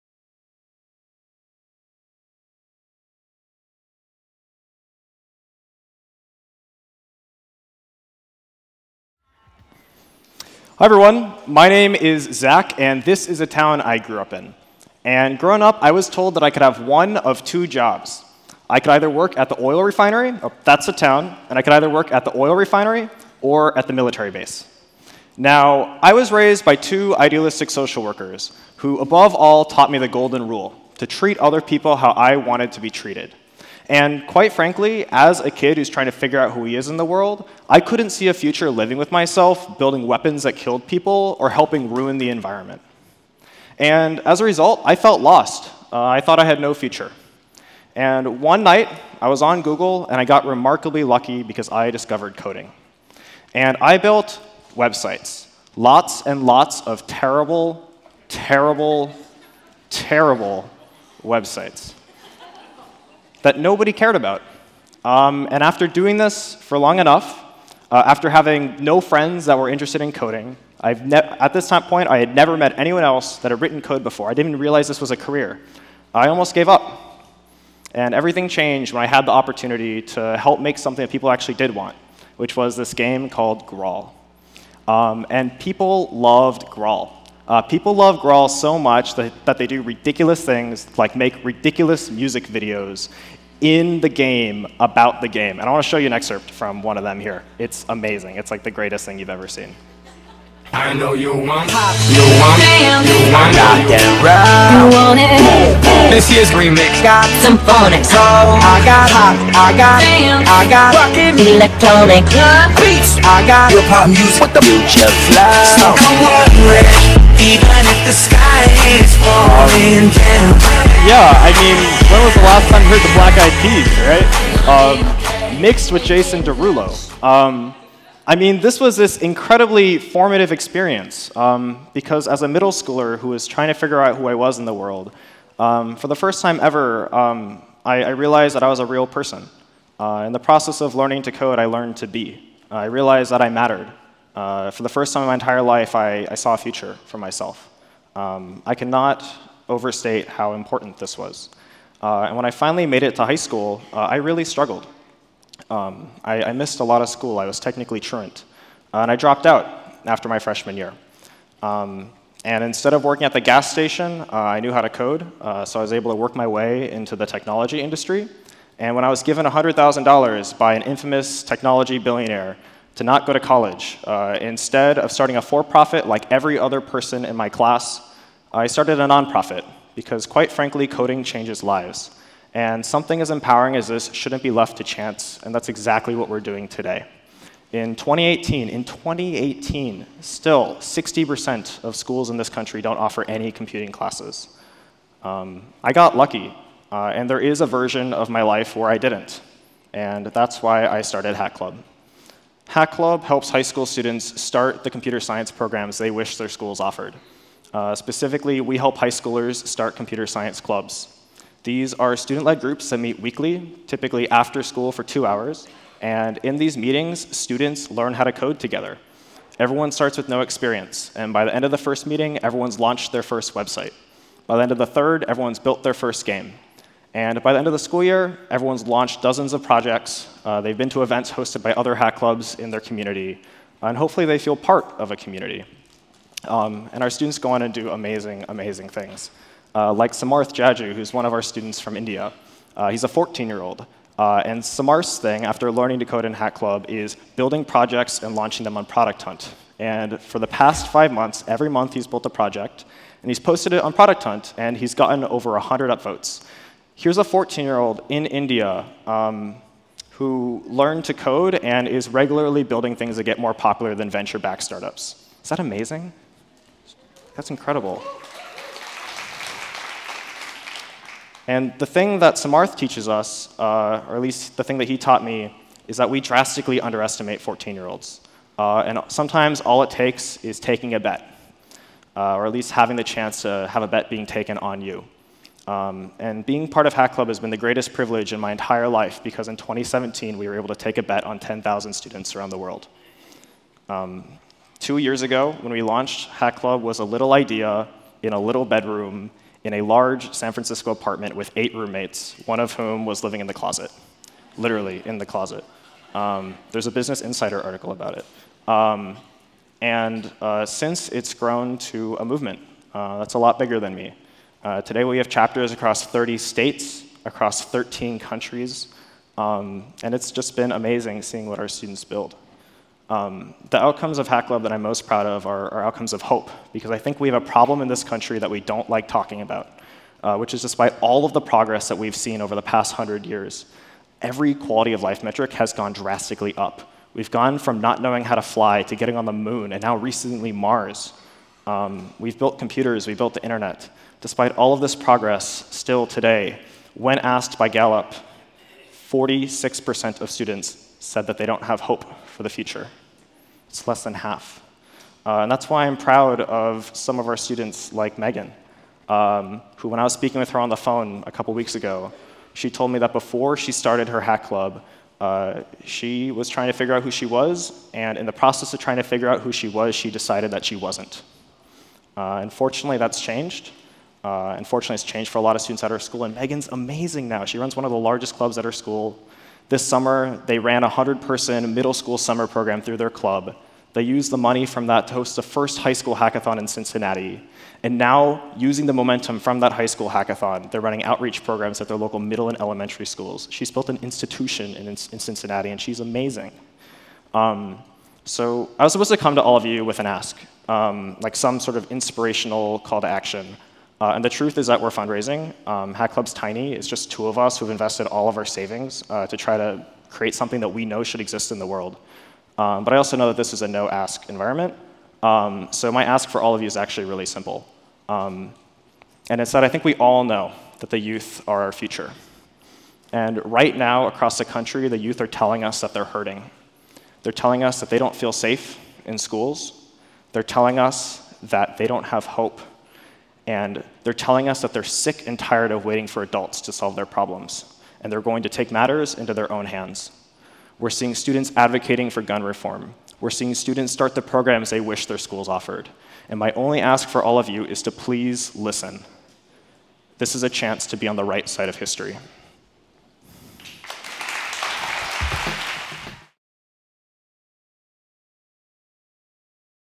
Zack Latta, Founder of Hack Club | AGG 2018
Zach Latta is the Founder & Executive Director of Hack Club, a platform enabling high schoolers to start awesome after-school coding clubs. Zach taught himself to code at an early age, and launched Hack Club to give all students access to this superpower. Hack Club is a Fast Forward Accelerator Alumni.